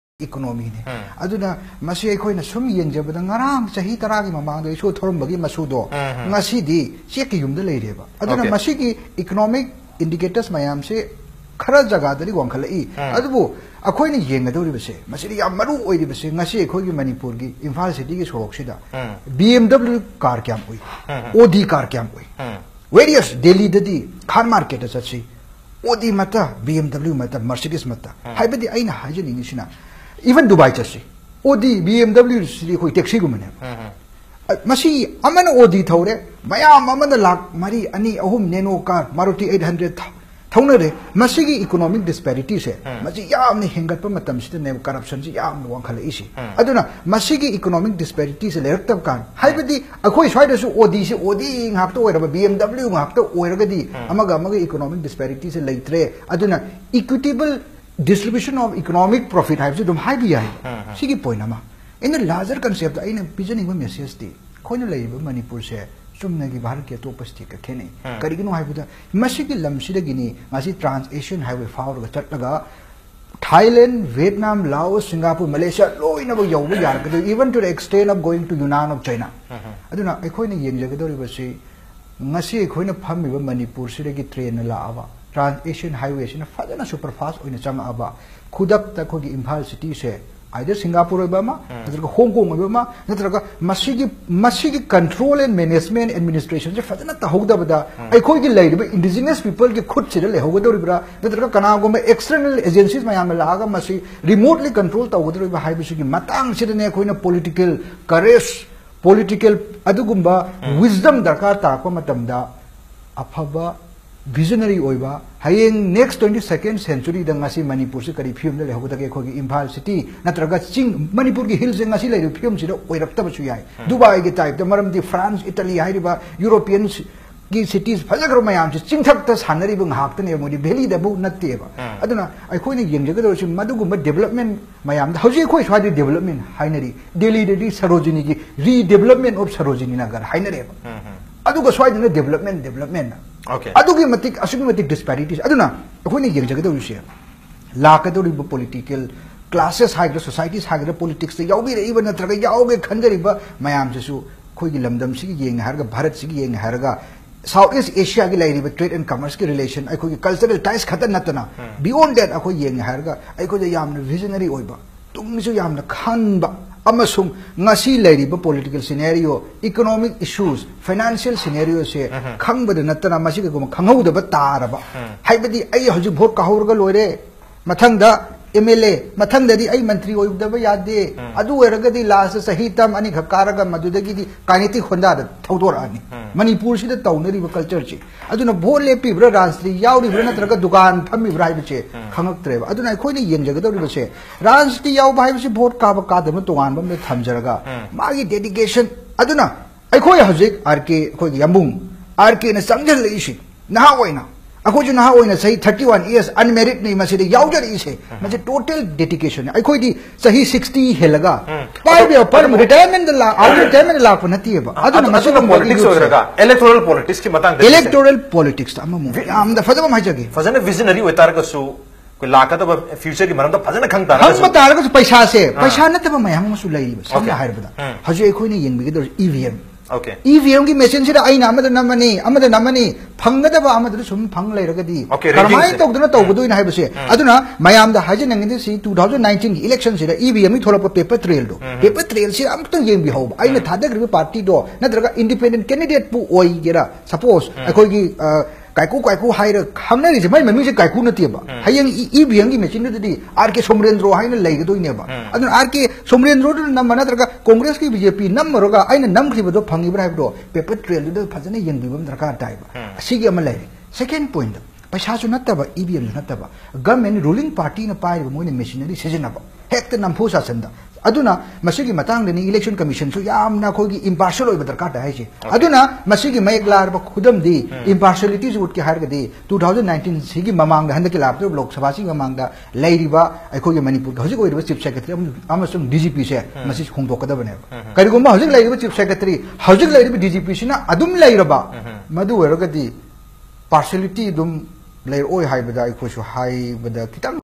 Ningombam Bupenda Meitei sharing his progressive and visionary views for Manipur, India and SE Asia
29th October, 2018, Imphal, Manipur